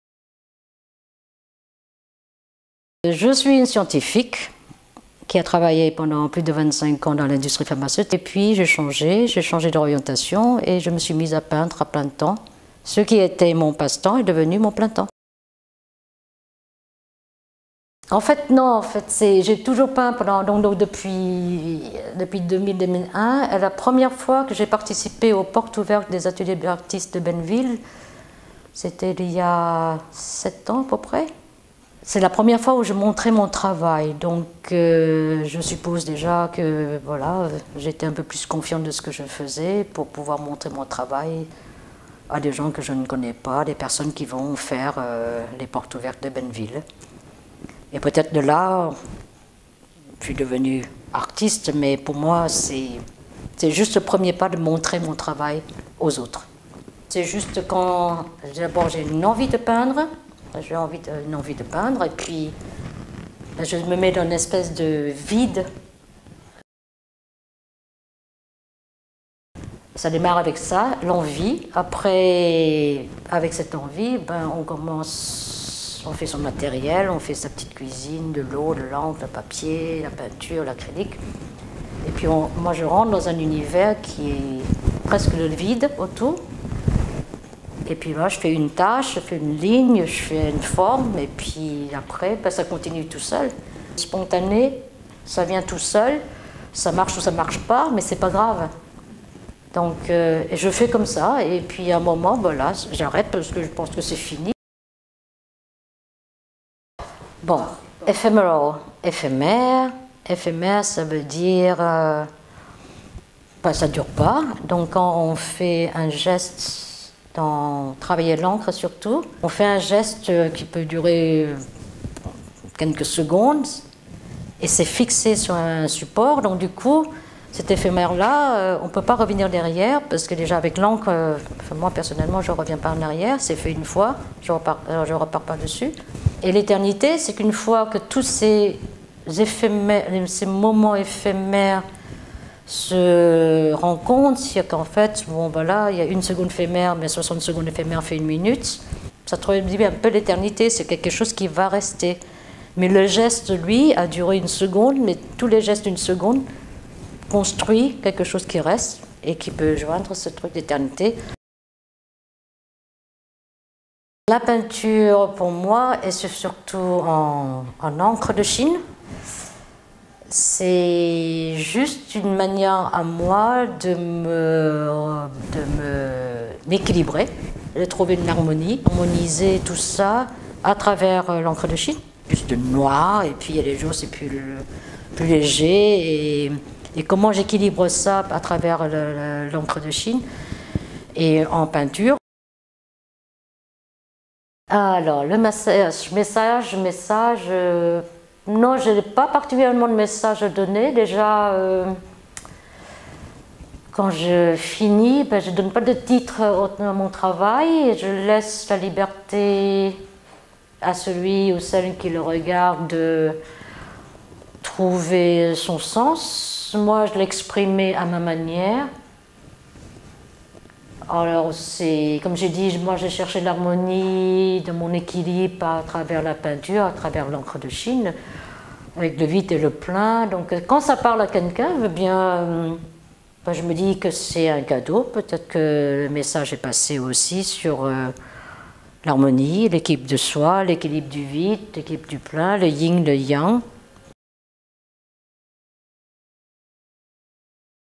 Paris Art Exhibition "Ephemeral Eternity" Artist Interview---Thu Huong Nguyen-Dufourmantelle-Eng
“Ephemeral Eternity” is an exhibition about experiencing the invisible passage of time through the chronoscope of an intimate dialogue between humans and nature. How can we understand the intervals of time within “Ephemerality and Eternity”? How can unexplainable perceptions of changes be transformed into a universal language of feelings? In this exhibition, nature is an active part symbolize the eternal life associated with the artist temporal body, the dialogue was draft from the essence of “breath”, which universal beings are shared equally, then flows to the body, the flesh and the fresh, it leaves an open ending with the formula of cancelling material information for the rebirth of interior forestation: the new language continues infinity conversations. Breath, body and language build up the tunnel to feel the sensitivity of time, beyond the present.